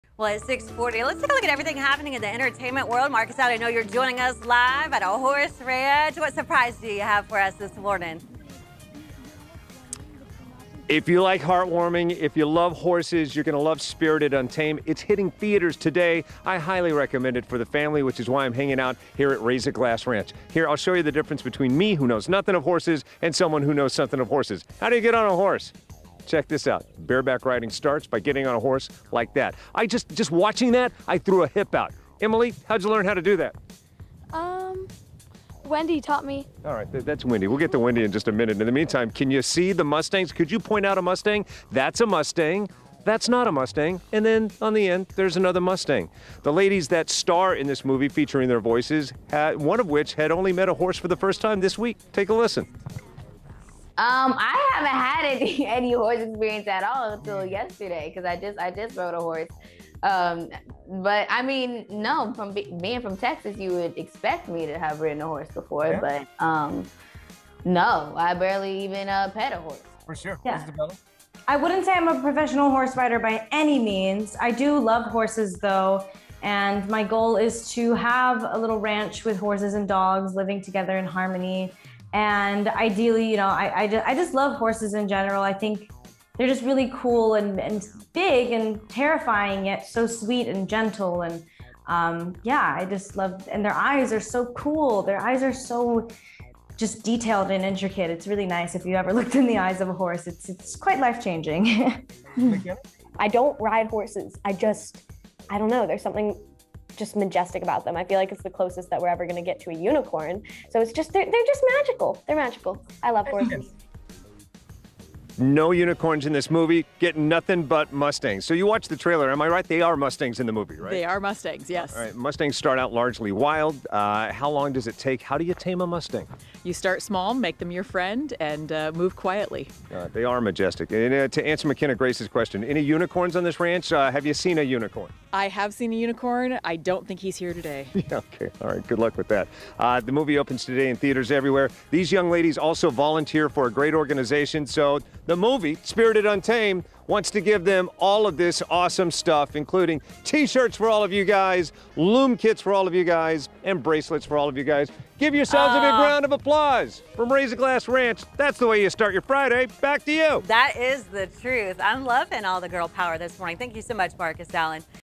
A look at new movie 'Spirit Untamed,' and Raise a Glass Ranch talks taming mustangs | Entertainment
From DreamWorks studio, 'Spirit Untamed' is in theaters on Friday.